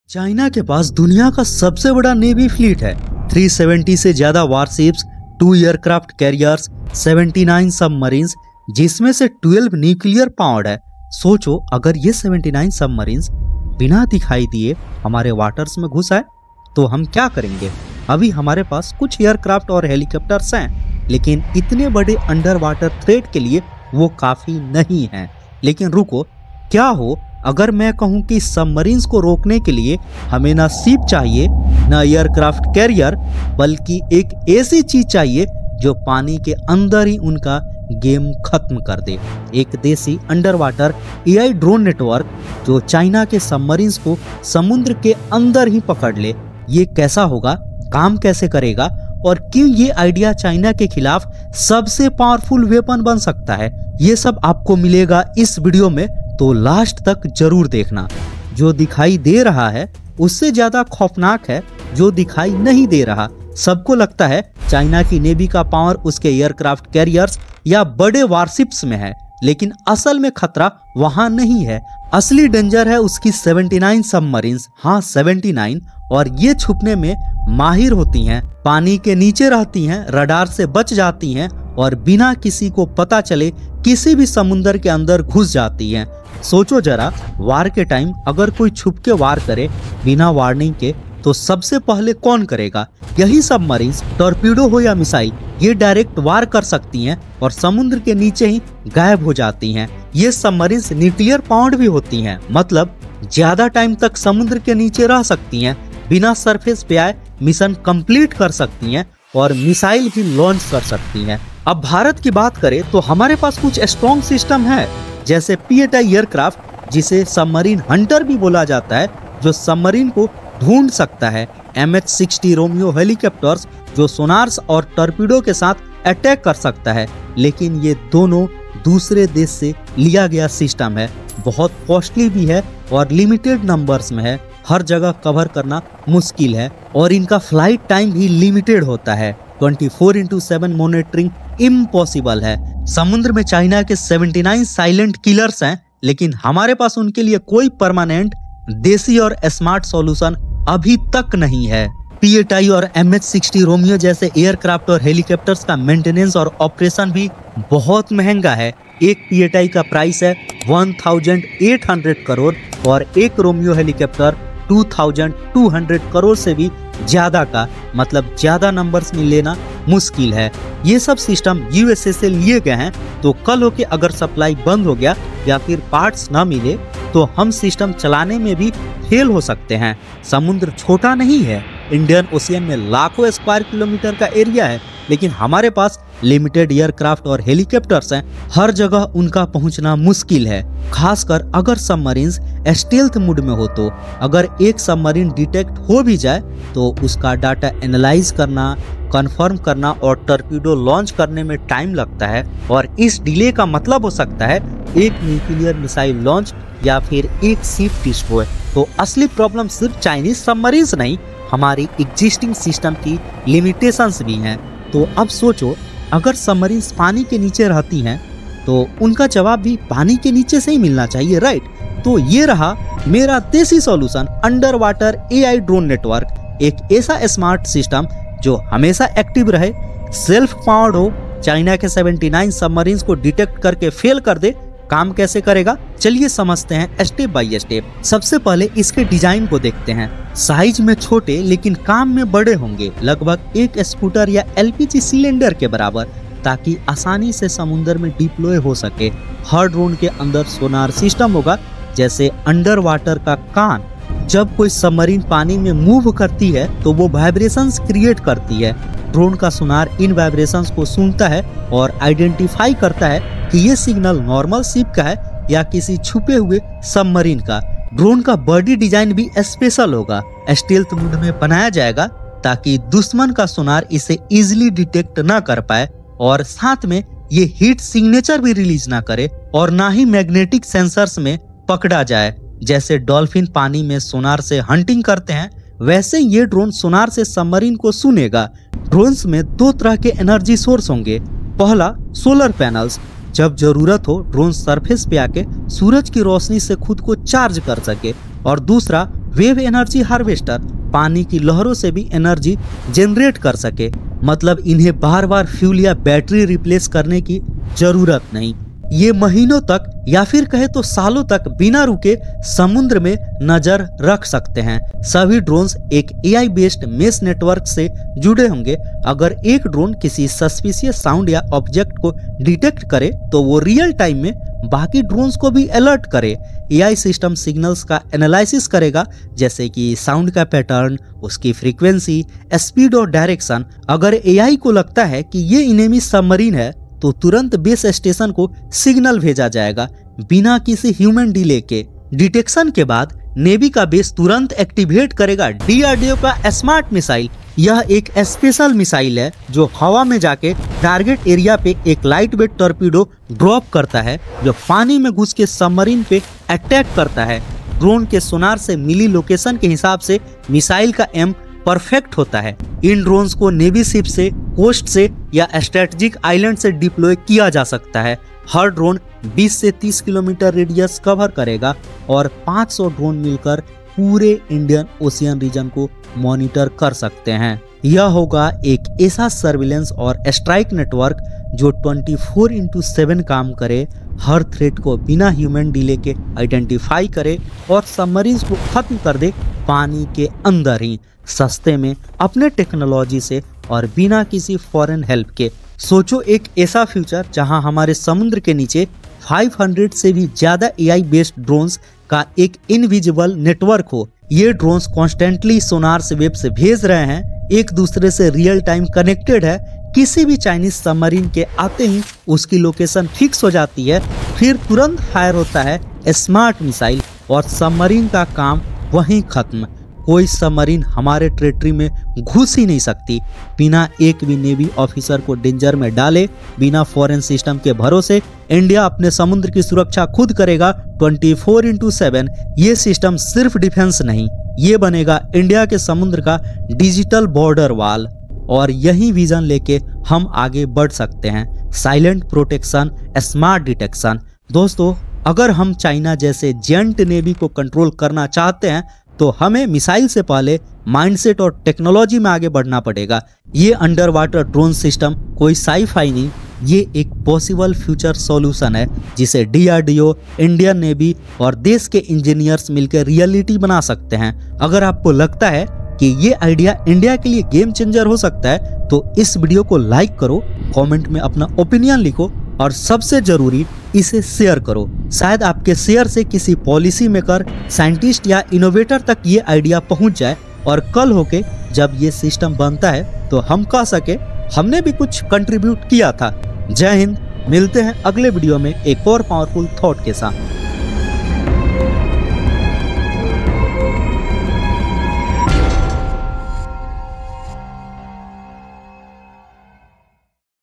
How India Stopped the World’s Largest Submarine Fleet? | Underwater Drones | India Vs China
How India Stopped the World’s Largest Chinese Submarine Fleet? | Underwater AI Drone Defence

India is building a game-changing defence system in the Indian Ocean — an AI-powered underwater drone network that never sleeps.
Powered by wave energy harvesters and solar panels, these drones are equipped with sonobuoys to detect enemy submarines in real time.

Once detected, they can instantly trigger DRDO’s SMART Missile for a lightning-fast strike — making it nearly impossible for Chinese submarines to escape.

In this video, we break down:

How underwater drones work in India’s defence

The role of wave energy & solar power in keeping them active 24/7

How sonobuoys detect submarines silently

DRDO’s SMART missile and its deadly reach

Why this system could change naval warfare in the Indian Ocean

🌊 This is the future of anti-submarine warfare — and India is ready.

Anup Missile – Where Curiosity Meets Clarity. India’s Vision, Explained.